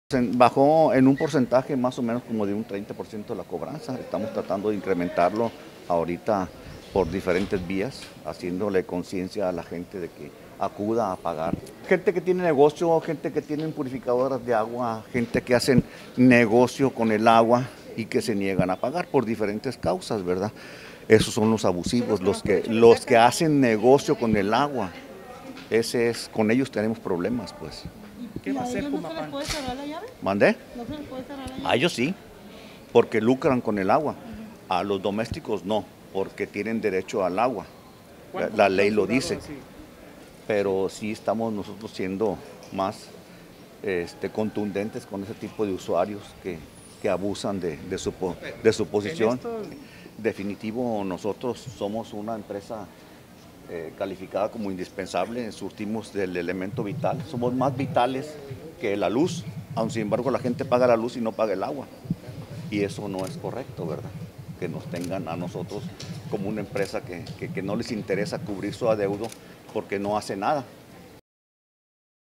Negocios Abusivos “Lucran con el Agua”: Jumapam
*Un 30 por ciento han dejado de pagar
*Se escudan en la contingencia sanitaria